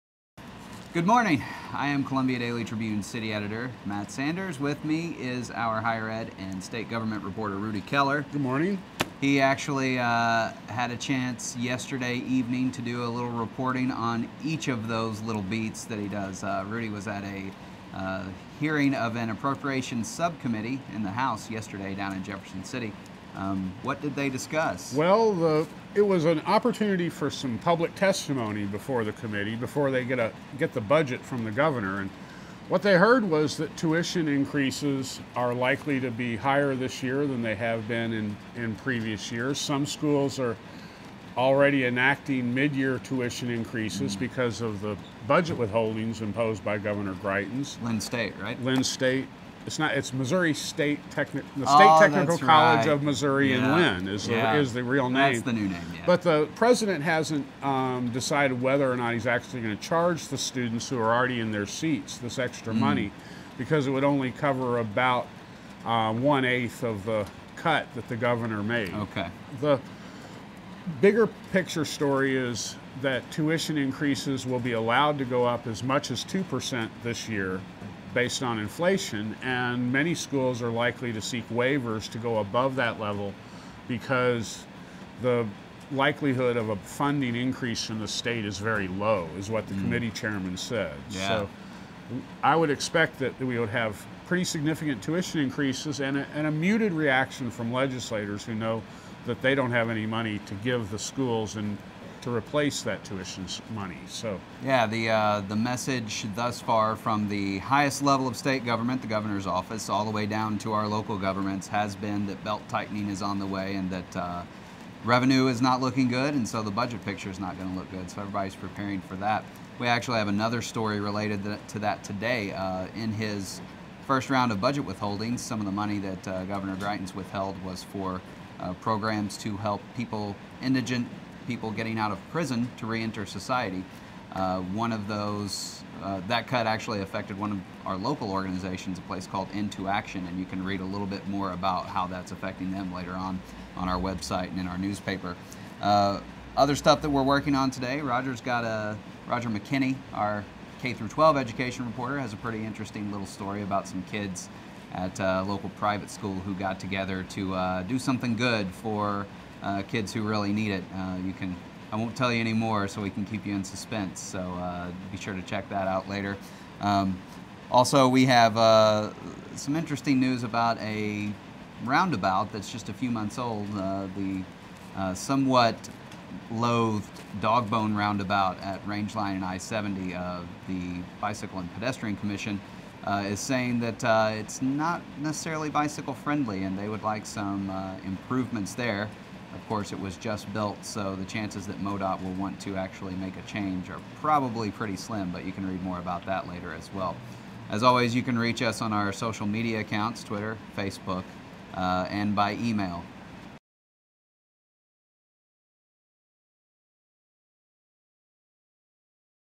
Tribune Daily Digest Tuesday, January 24, 2017
Rudi joins the digest to talk about likely tuition increases at Missouri colleges and universities. And Matt teases stories today about kids going out of their way to help other kids and complaints about the dogbone roundabout.